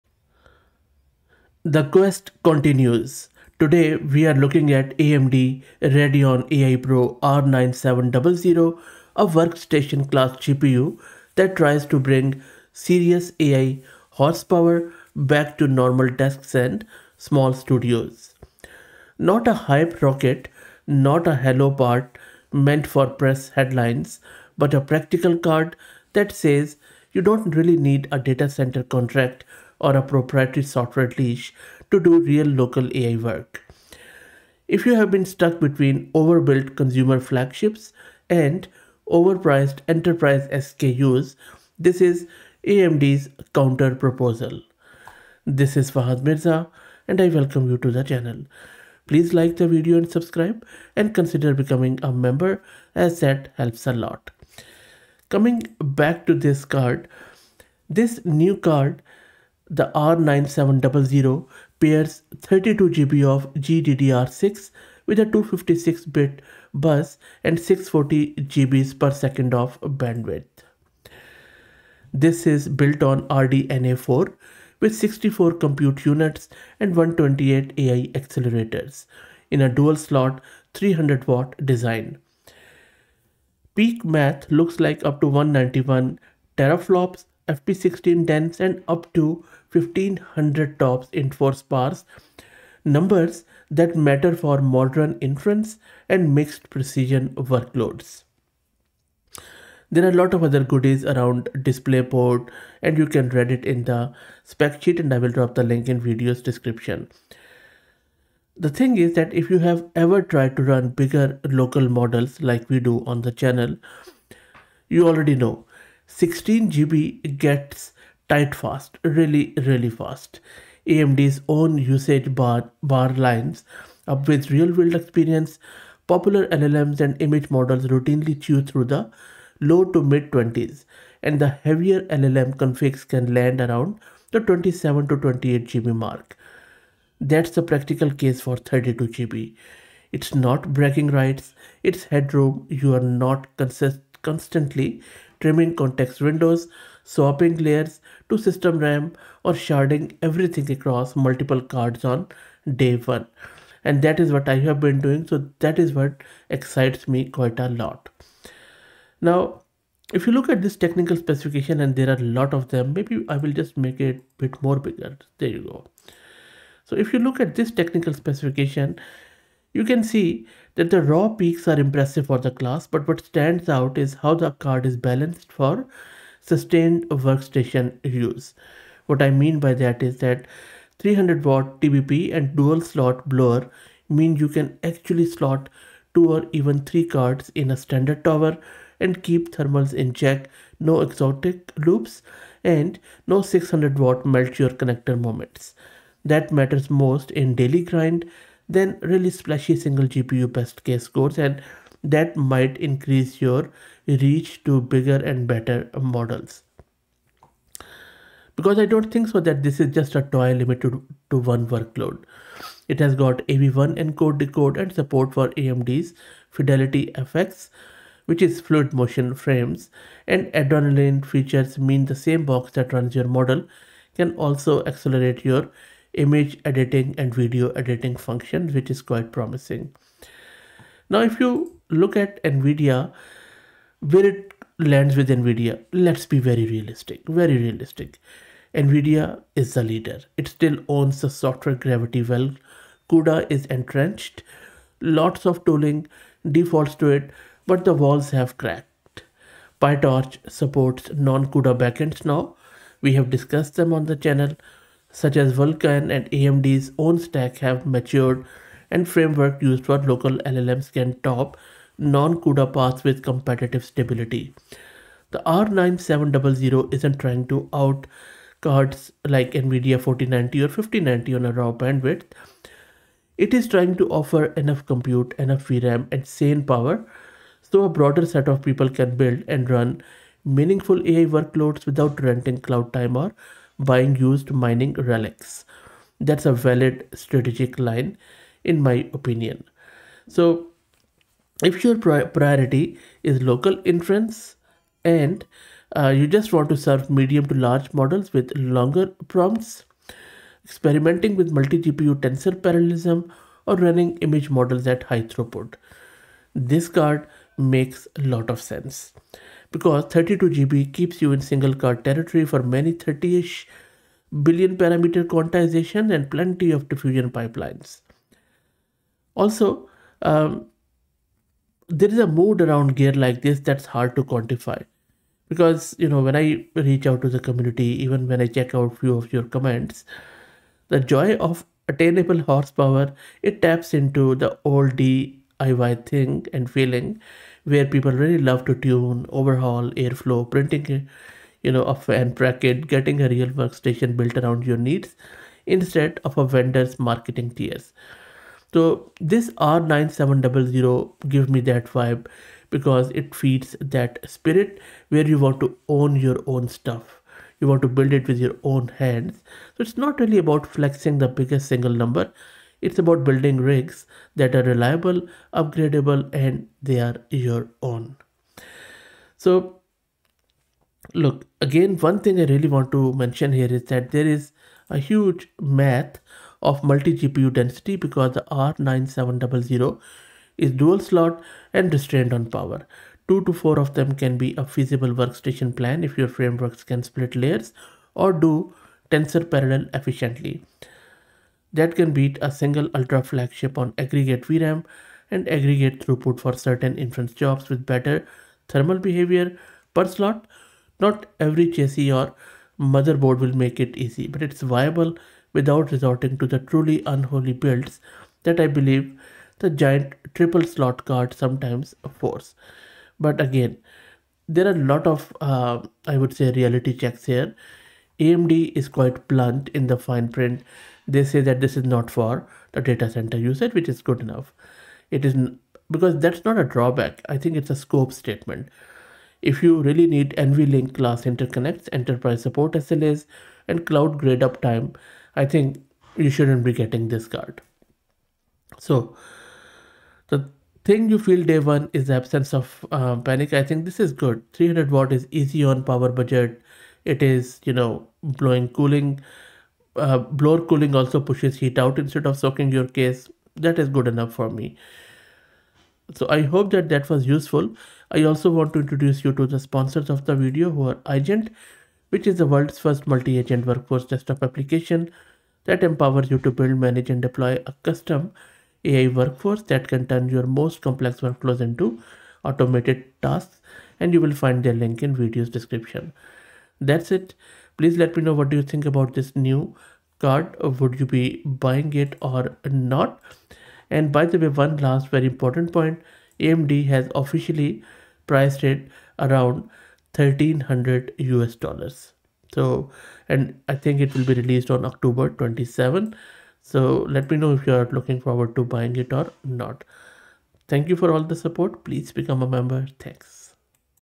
AMD Radeon AI PRO R9700 Drops Oct 27 for $1299 – 32GB Monster Incoming!
A brutally honest look at AMD’s Radeon AI PRO R9700—with real talk on 32GB VRAM, performance, and whether it dents Nvidia’s lead.